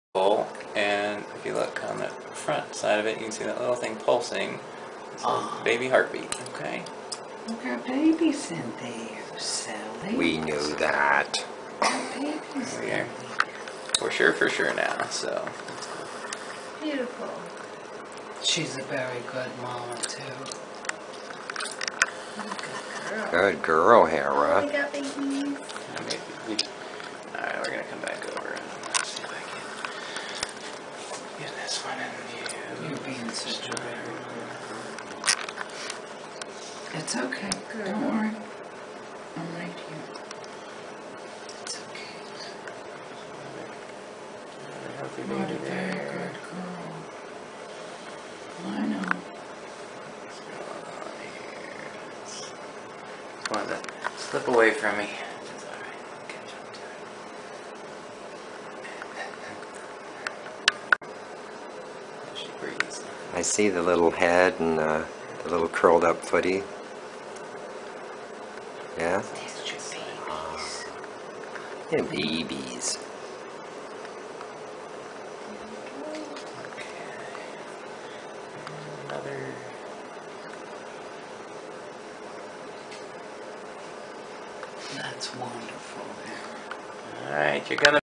Hera's Bullmastiff Puppy Sonogram
Hera's Sonogram on July 3rd, 2018 at the Animal Medical Clinic with Dr. Young and Vet Tech Rebecca & Gamma Bianca.  Lots of babies !!!